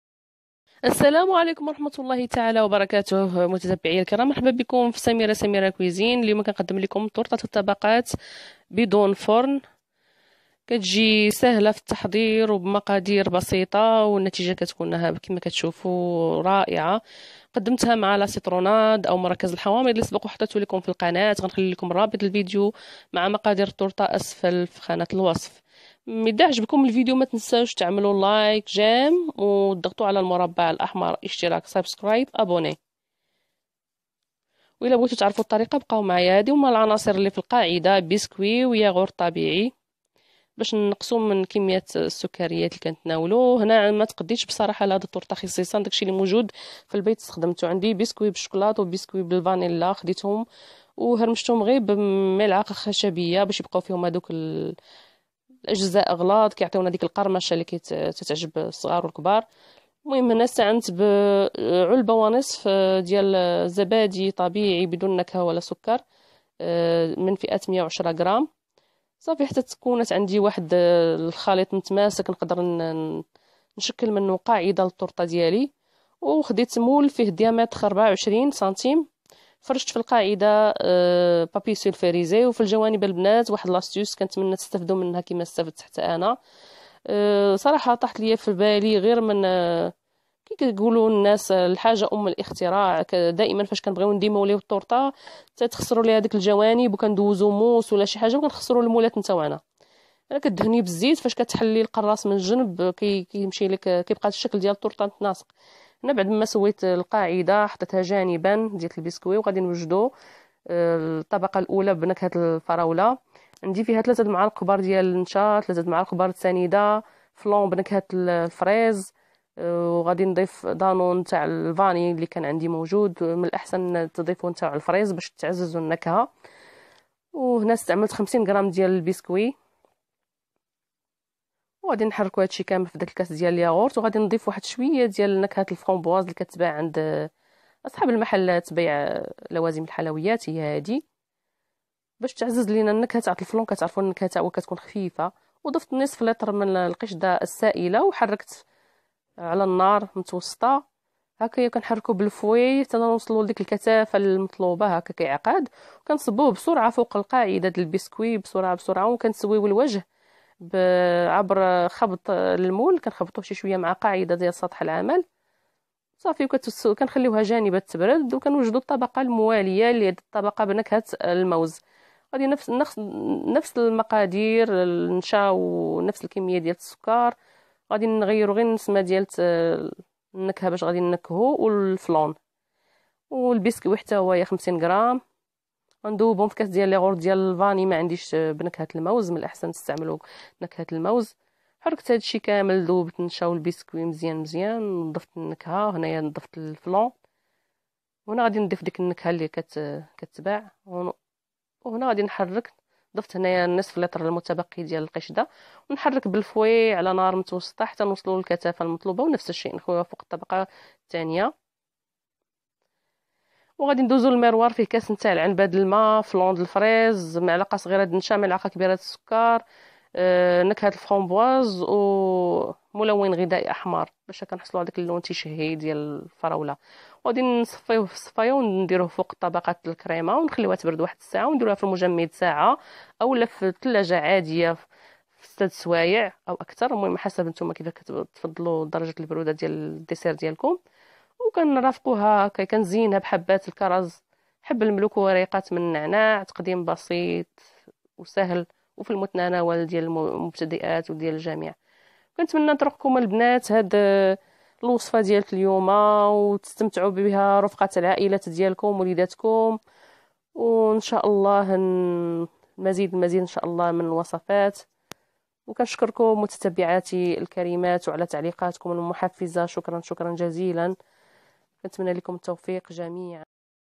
طورطة منعشة لصيف حار 🌅بدون فرن/بدون بيض/بدون جيلاتين وبمذاق 🍓و🍌اكثر من روعة ومكونات كانت بالبيث.
طبقة البسكوت :اي نوع بسكوت 300g+ياغورت طبيعي علبة ونصف من فئة 110g. طبقة الكريما بنكهة الفراولة : نصف لتر قشدة طرية و 50gبسكوت اي نوع من الاحسن استبعاد الشكلاطة المحافظة على لون الوردي زاهي وتختار بسكوت ابيض "فانيلا "مثلا و3معالق كبيرة سكر حبيبات و3معالق كبيرة نشا أو مايزينا و ياغورت فاني ومن الاحسن فراولة لتعزيز نكهة الفراولة فالتحلية .فلان فريز "فراولة"ونكهة الفخومبواز "توت احمر "ونخلط جيدا حتي تنسجم العناصر ونضيف القشدة السائلة و نضع الكل على النار مع التحريك المستمر طبقة الكريما بنكهة الموز : نصف لتر قشدة طرية و 50gبسكوت فاني و 3معالق كبيرة سكر حبيبات و3معالق كبيرة نشا أو مايزينا وياغورت فاني ومن الاحسن يكون بنكهة الموز ونكهة الفانيلا أو نكهة الموز لاثنين مناسبين فالتحلية ..وفلان بنكهة الموز .و ونخلط جيدا حتي تنسجم العناصر ونضيف القشدة السائلة وعلى نار متوسطة مع التحريك المستمر حتى الحصول على الكثافة المطلوبة الطبقة الحمراء اللامعة :كاس شاي "عنبة "ماء عادي و ملعقة كبيرة سكر حبيبات و ملعقة صغيرة نشا أو مايزينا و فلان بنكهة​ الفراولة و نكهة الفخومبواز المركزة ..و القليل من الملون الغذائي احمر و نغلي الكل فكاسرول على نار متوسطة وفوىىالغليان يصفي ويسكب على وجه الطورطة ويبقى جانبا حتي يبرد و بعد ذلك ندخل الطورطة لتبرد جيدا أما فالثلاجة 6ساعات أو 1ساعة فالمجمد و تقدم بارد وبالصحة والراحة